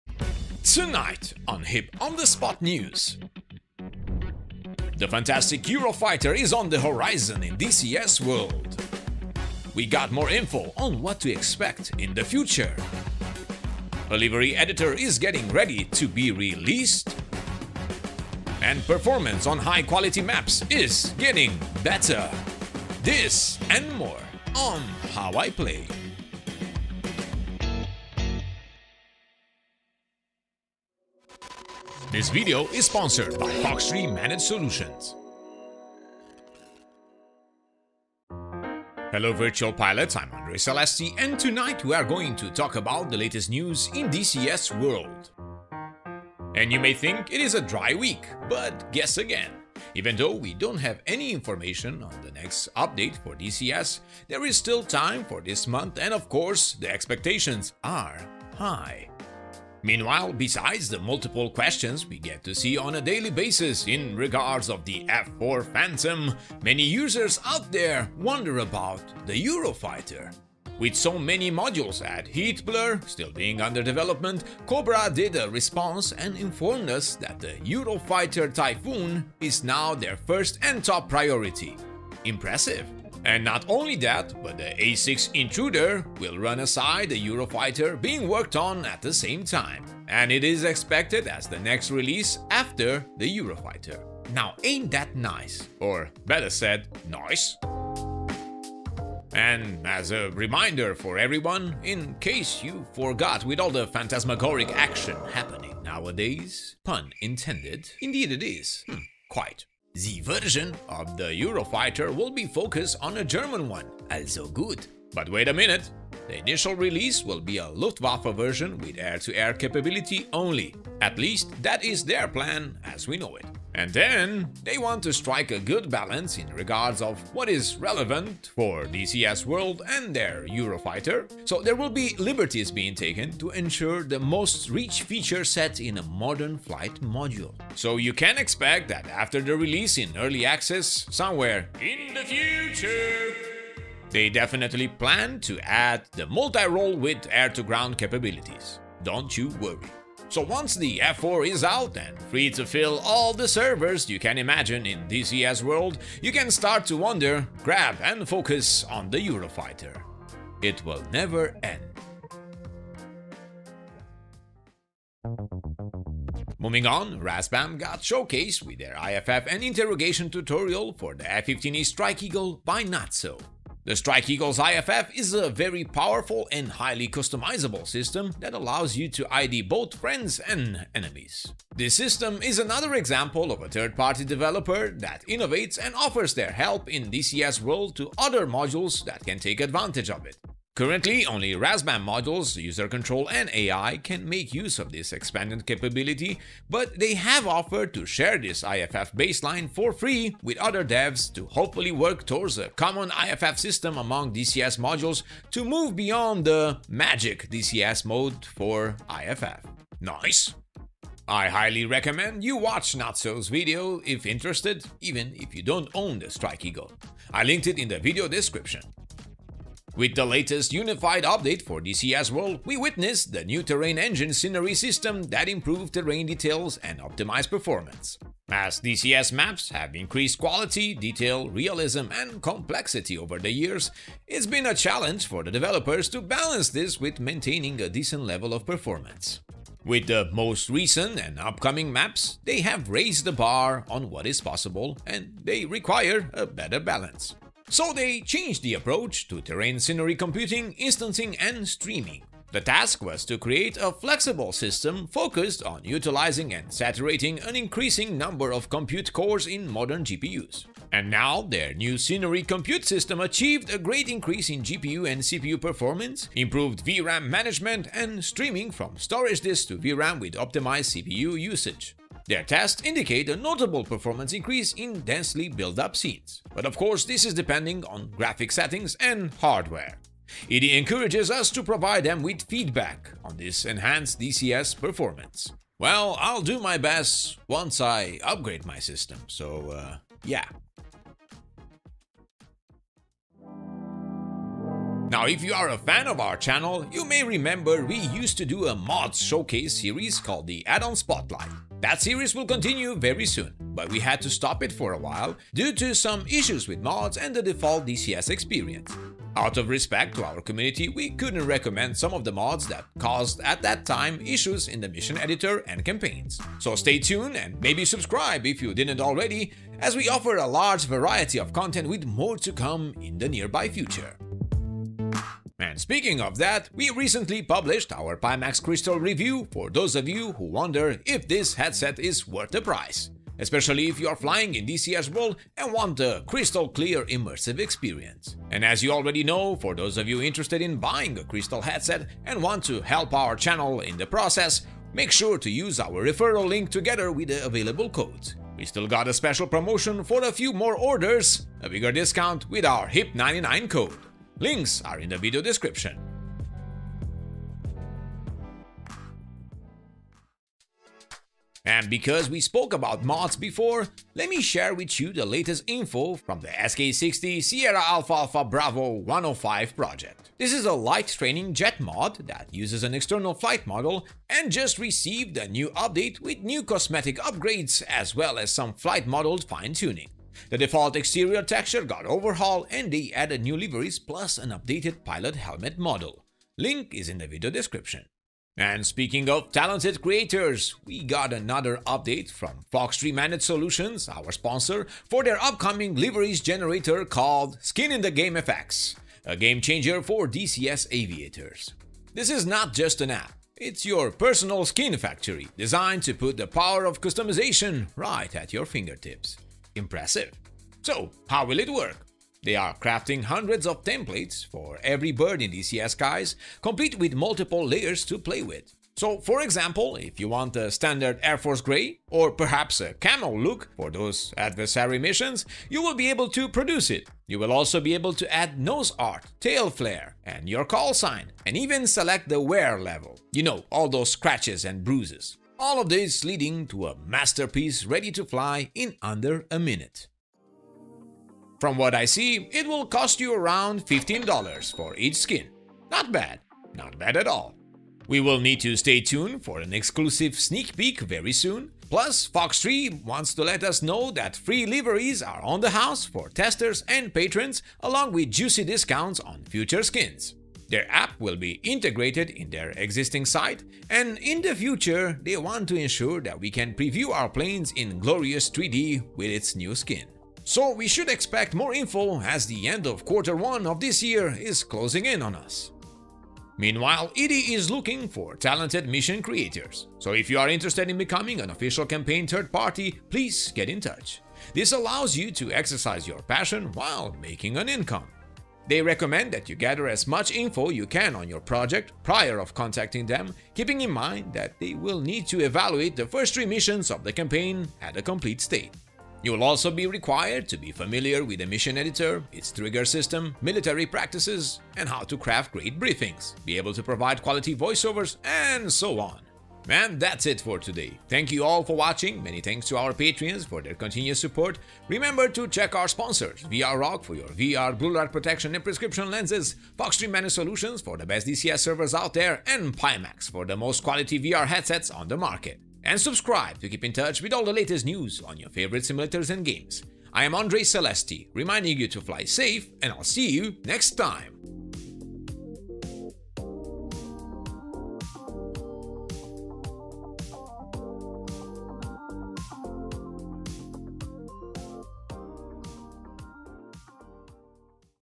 DCS Eurofighter Info | Version & EA | Livery Editor | A6 Intruder | F15E IFF | Maps FPS Boost
We got more news about the upcoming EF200 and A6 Intruder EA plan by Heatblur & TrueGrit, a Livery Editor by Fox3 Managed Solutions and current progress on Performance improvements for DCS maps
- use the LIMITED CODE - hip99 - for the next 10 orders and get $99 USD off.

Thumbnail art by Trustii - Official DCS Screenshot Artist using the EF2000 VSN Mod